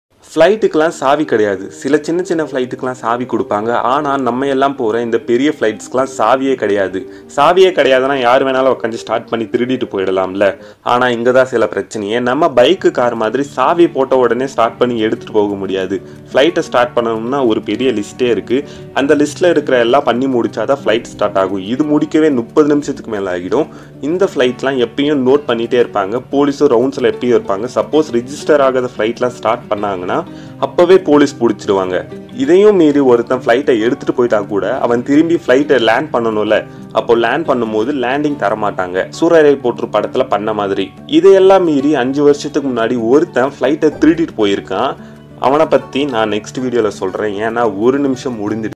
ennathu flight ல savi கேடியதா 😮
The Surprising Truth About Airplanes: There's No Key

Description: Have you ever wondered how pilots start an airplane engine? It's not with a key! In fact, there's no key at all! In this video, we're going to take a look at the surprising truth about airplanes and how they start without a key.